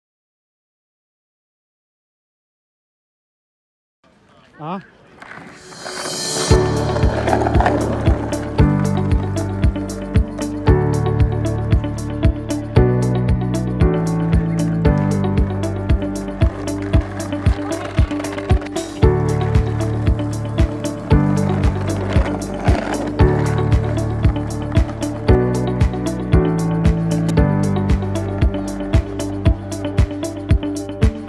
Skateboarding in Sweden 2020
Doing some skating on her new board